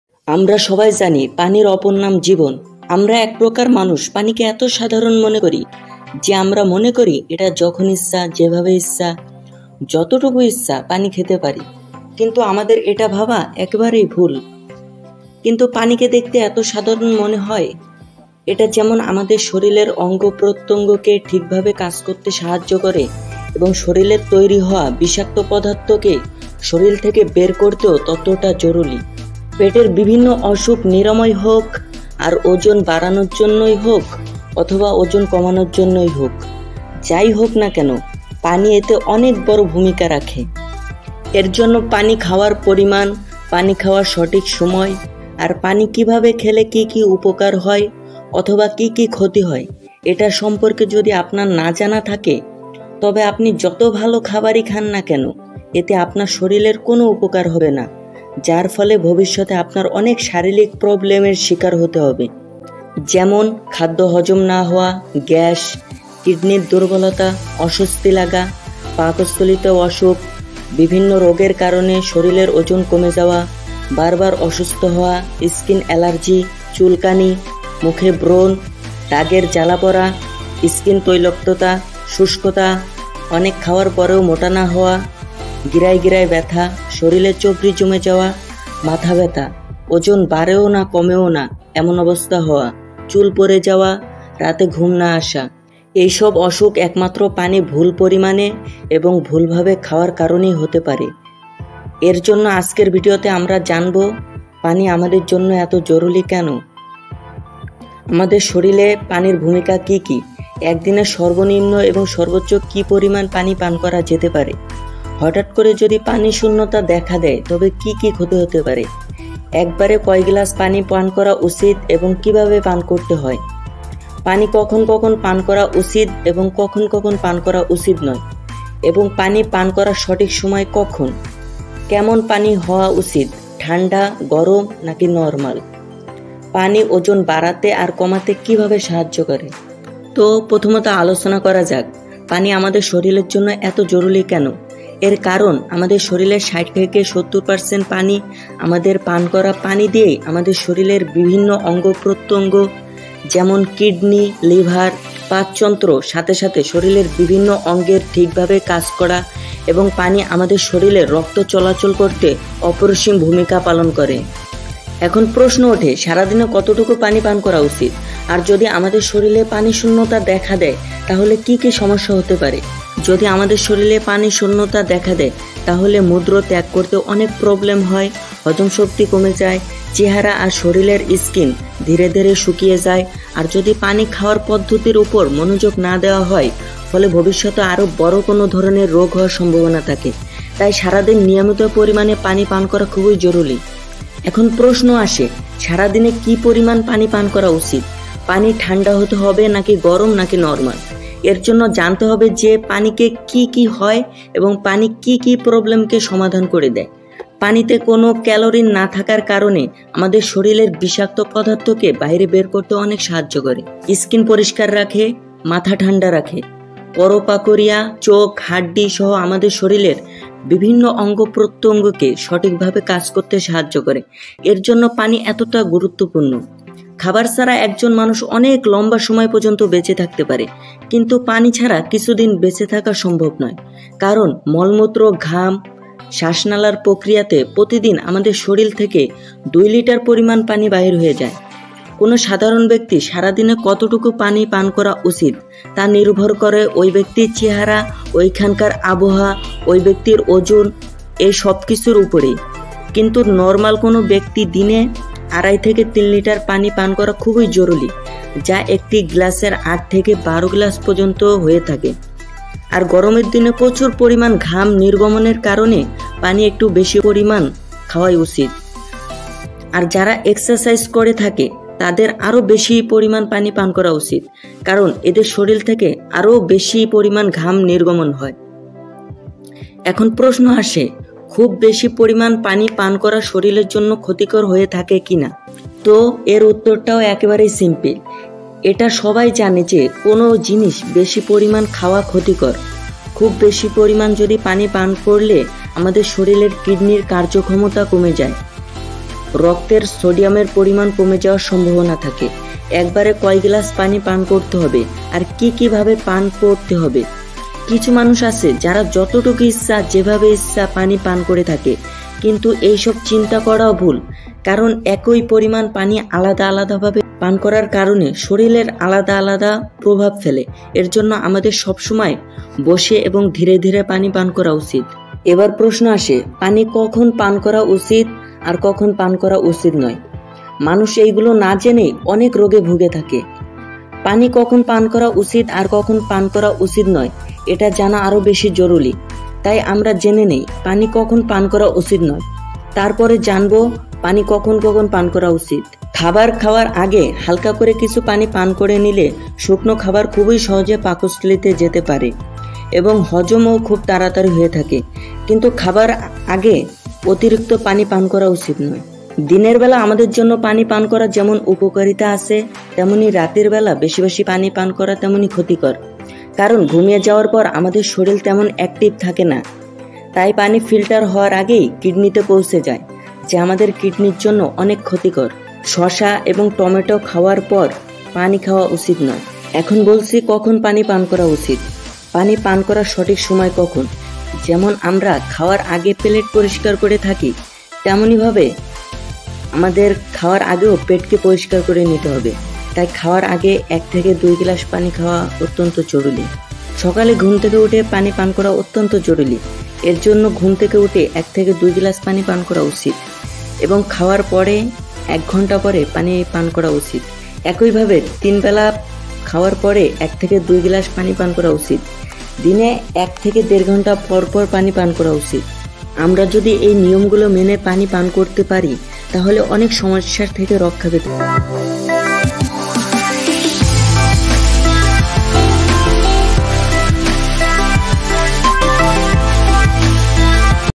পানির উপকারিতা লক্ষন, এবং পানির ক্ষতির লক্ষণ, পানি খাওয়ার উপর মনোযোগি হওয়া উচিৎ @mayajaalbangla Emu
পানির উপকারিতা লক্ষন ll এবং পানির ক্ষতির লক্ষণ ll পানি খাওয়ার উপর মনোযোগি হওয়া উচিৎ ll Emu official